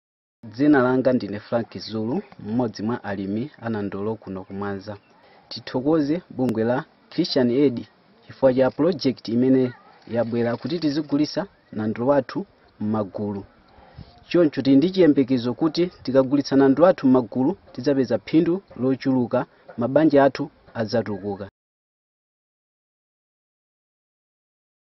A message from Frank to our Harvest Appeal supporters
Pigeon pea farmer, Frank Zulu, sends a message of thanks from Malawi.